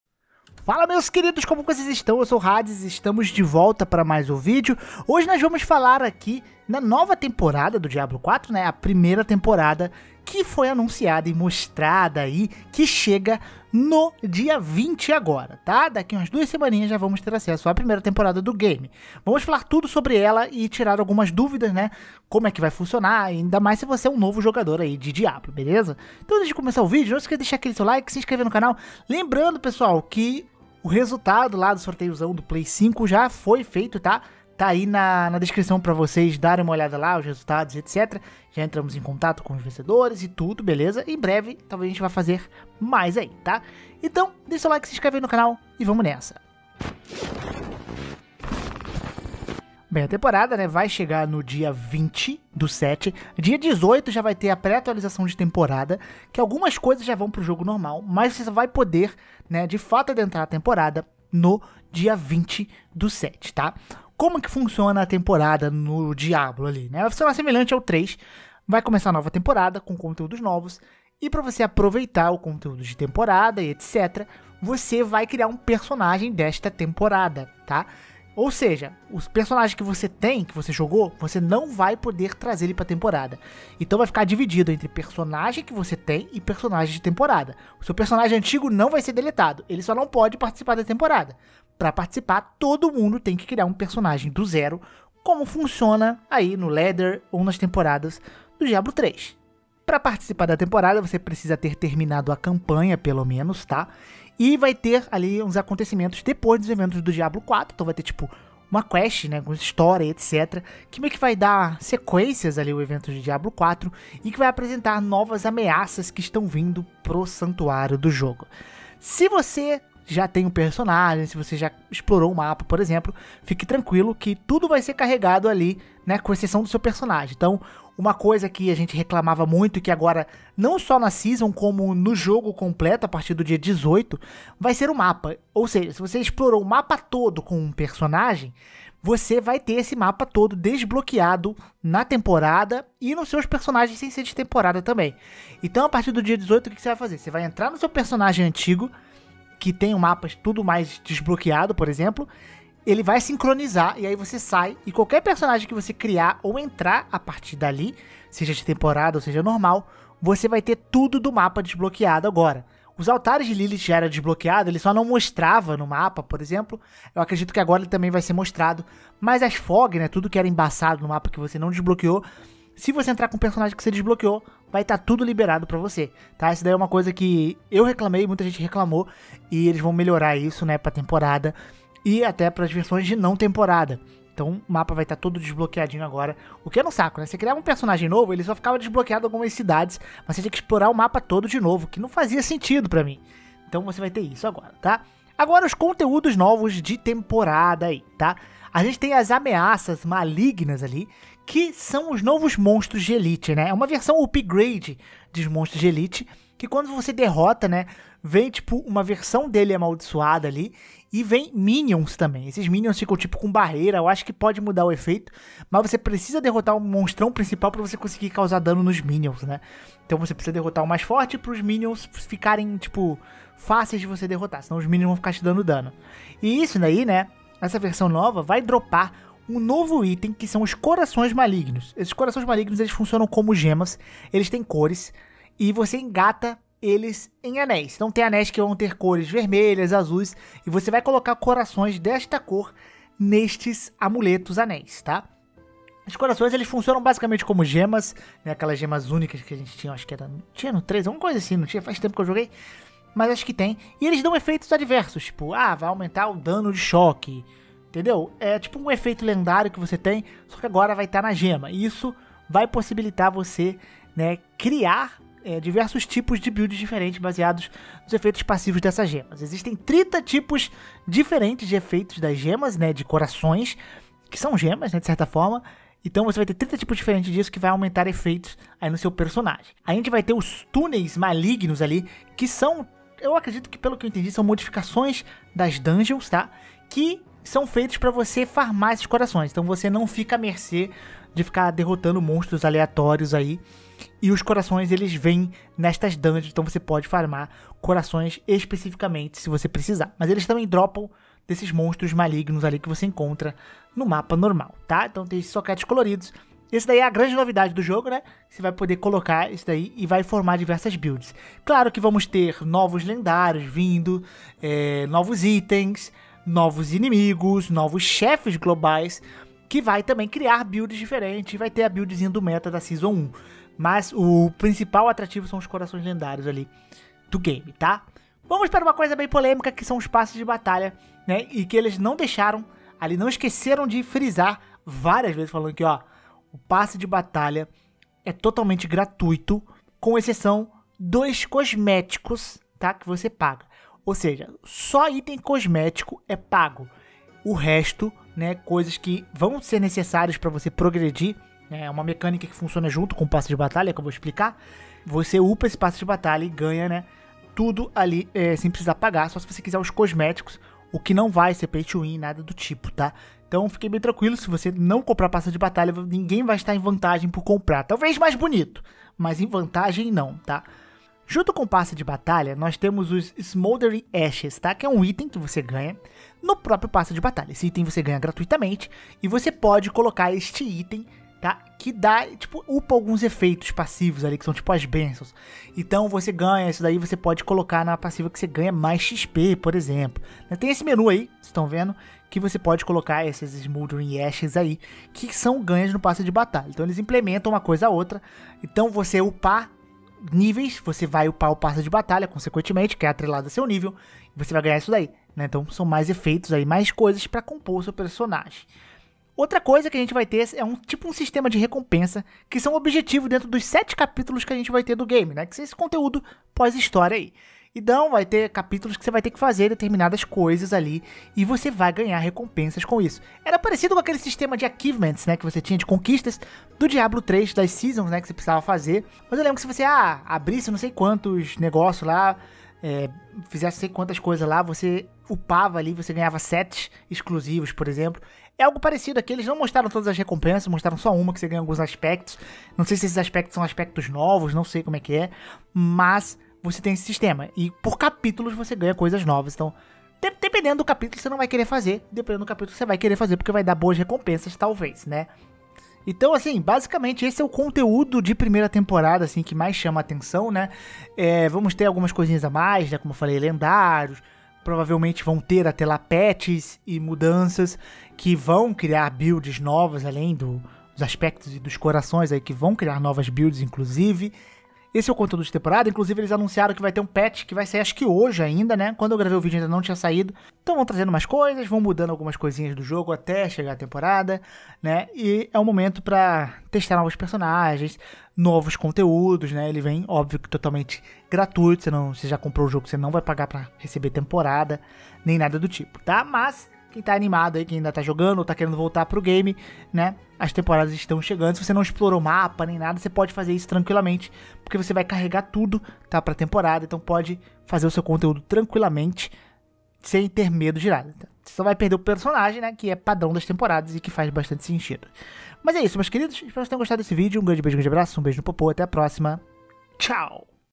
Tudo sobre a NOVA TEMPORADA de Diablo 4 que você PRECISA SABER! (Data, Mapa, Itens e Conteúdo novo)
A nova temporada de diablo 4 chega dia 20/07

➤ PSN: hadessama94

00:00 Temporada 1 como funciona
02:00 Mapa e Renome vai carregar?
03:15 Novos conteudos
05:30 Passe de batalha
06:49 Sistema de Recompensa